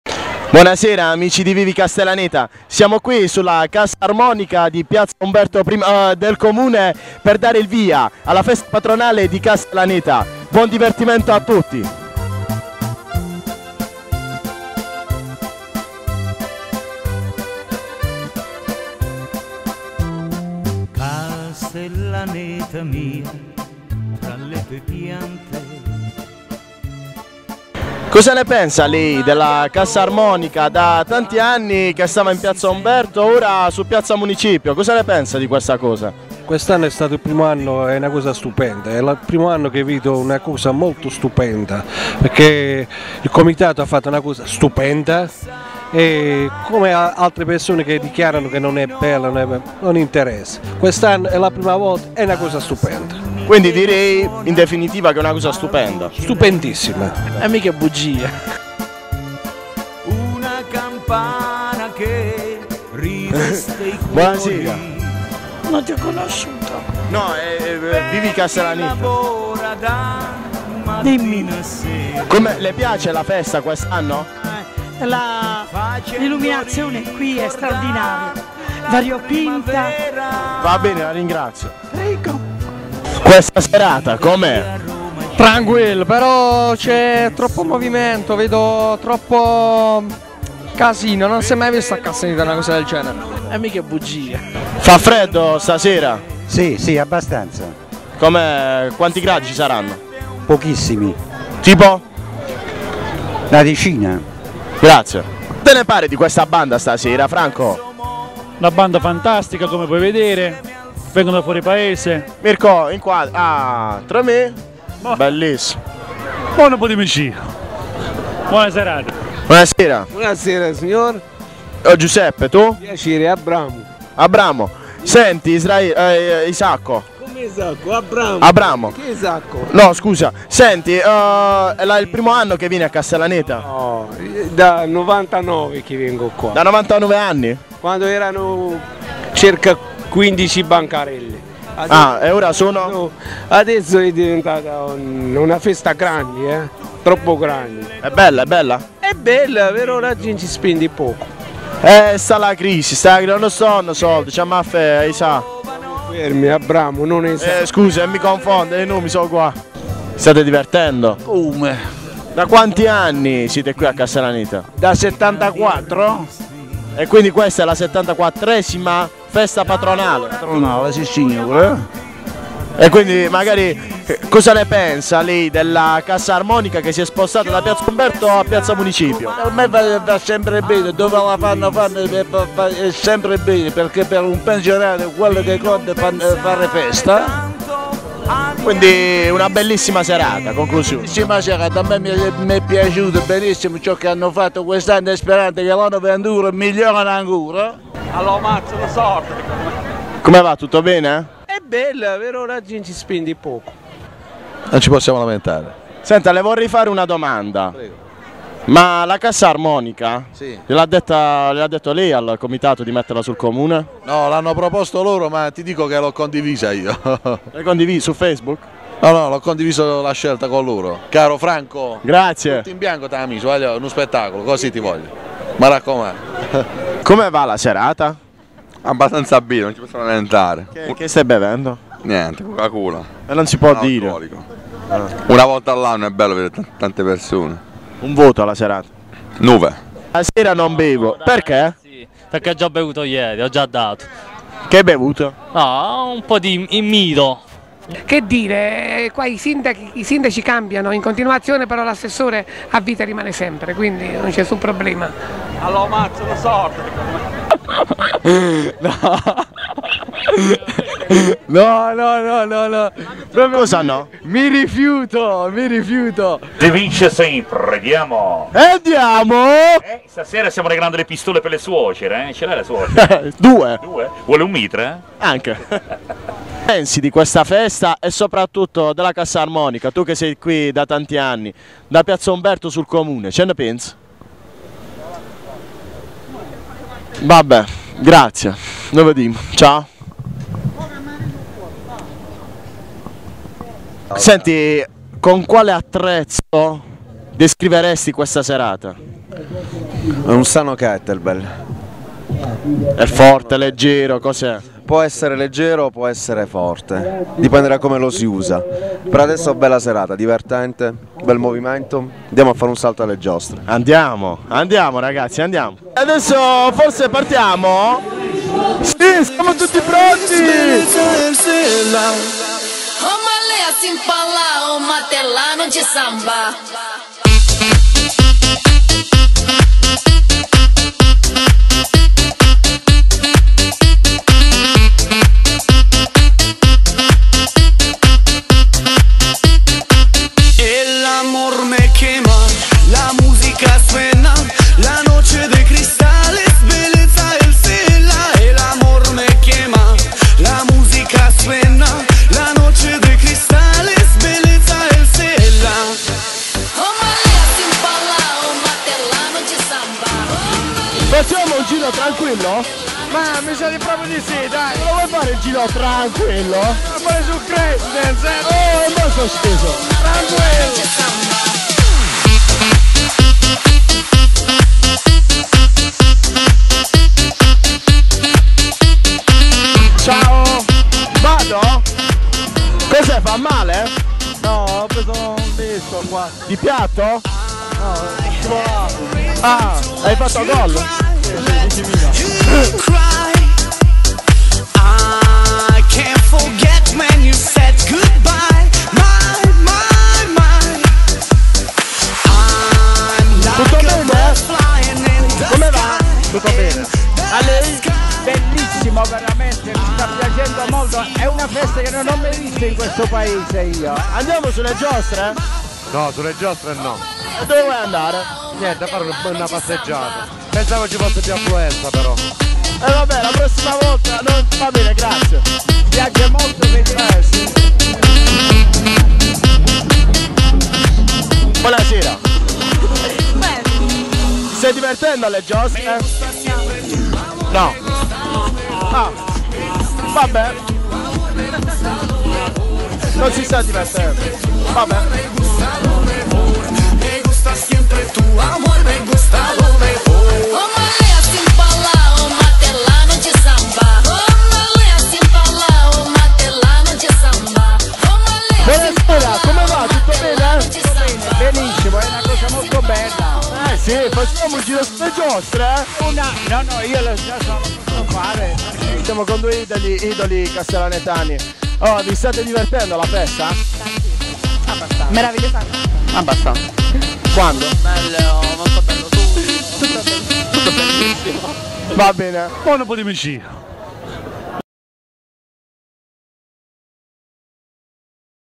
QUELLI CHE...LA FESTA PATRONALE DI CASTELLANETA · 11.05.2013
L'irruzione di ViViCastellaneta per le vie della città durante i giorni di festa patronale.
Come l'avranno presa i castellanetani ? Un video da vivere!